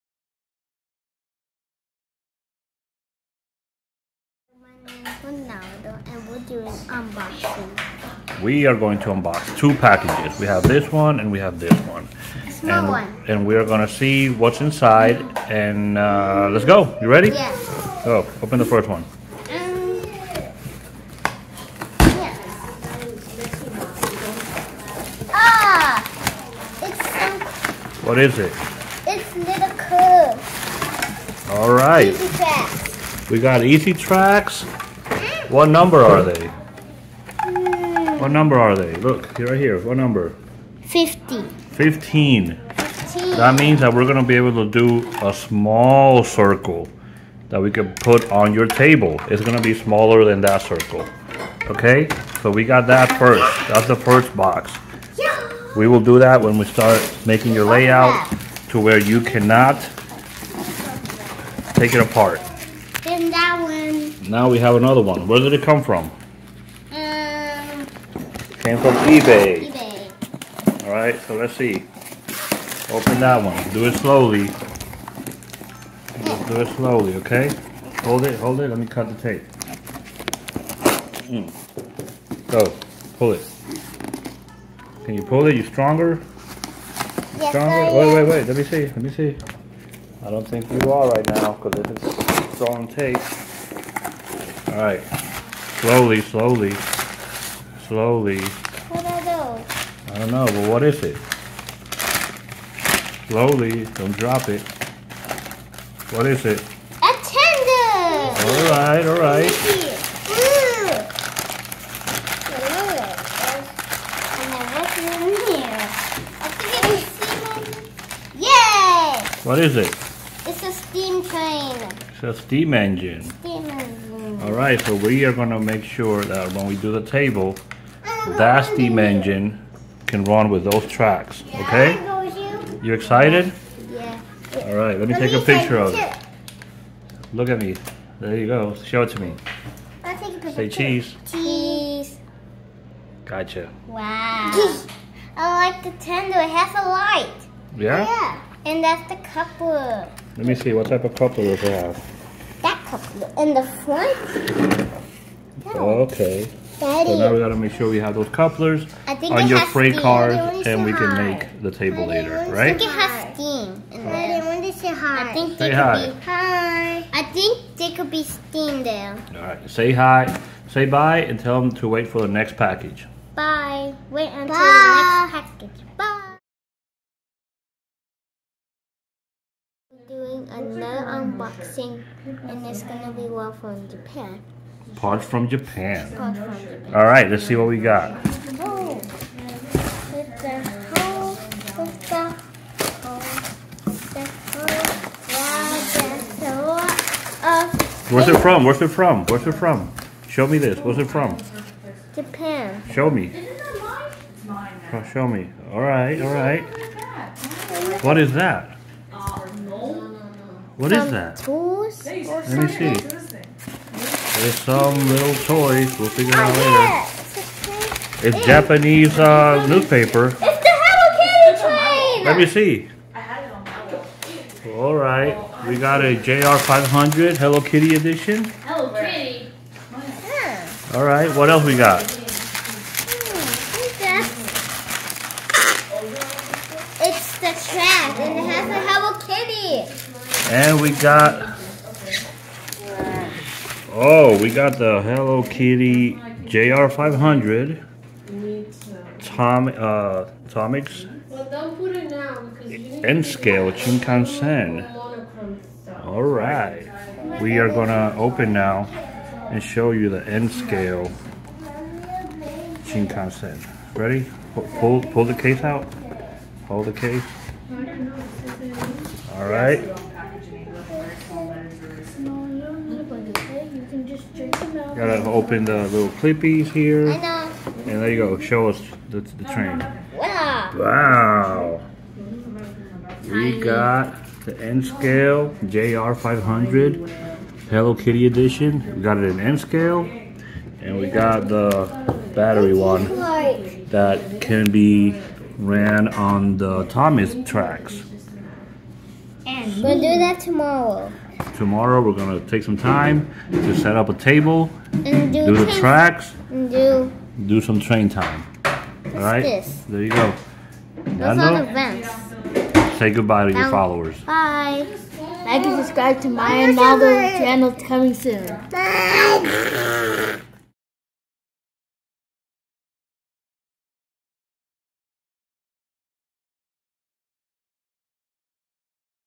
TRAINS: Ronaldo unboxing our 1st N scale. JR500 (Hello Kitty).
We unbox some trains!